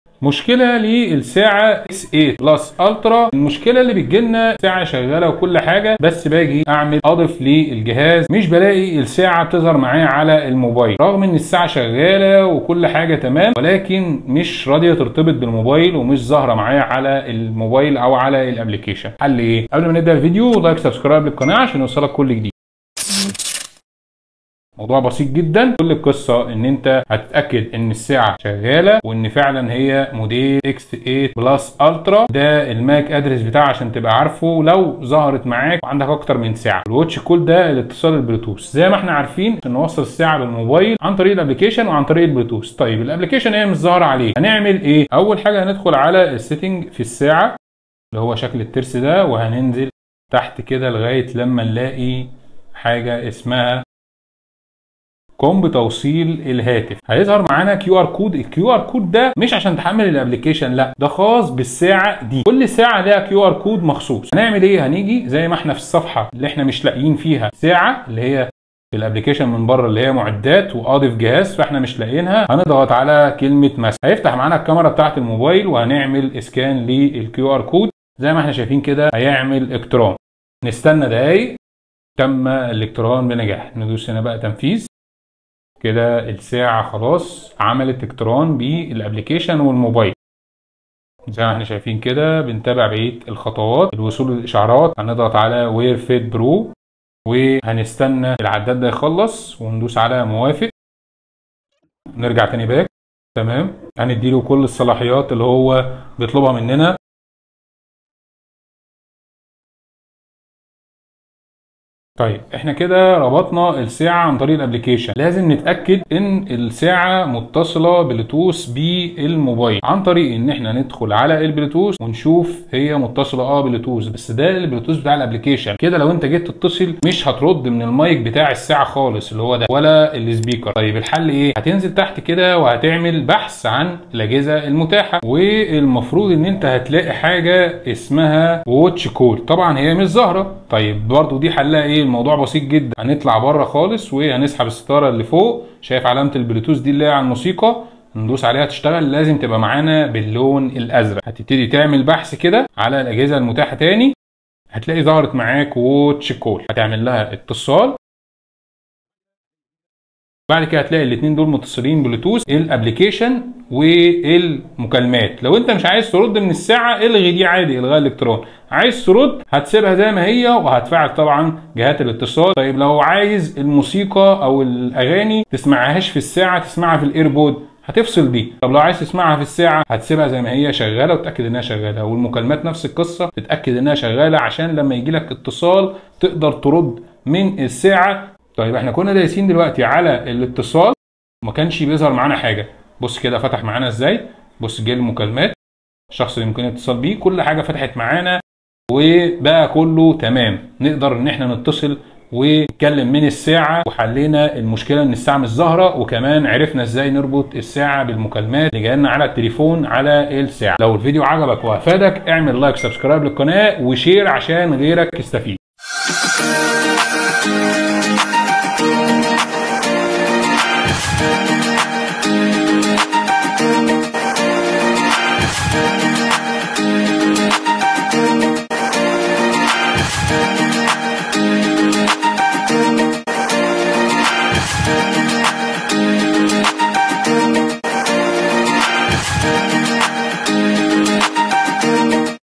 ازاي تظهر ساعة الترا X8+ Ultra في التطبيق؟Wearfit Pro
مشكله عدم ظهور الساعه X8+ Ultra  في التطبيق وحلها
لشراء الساعه باقل سعر في مصر
رابط الشراء
البيع اون لاين الدفع بعد الاستلام ومراجعة المنتج 💪